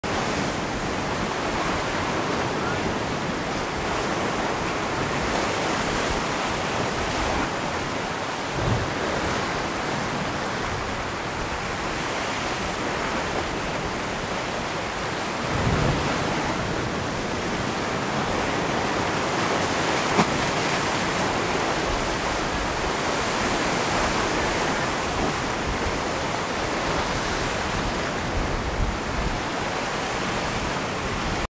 Churning, splashing waves in the Cape Perpetua Scenic Area of the Central Oregon Coast.
A sunny day in Central Oregon shines down on surging waves, rushing through Cook's Chasm at Cape Perpetua.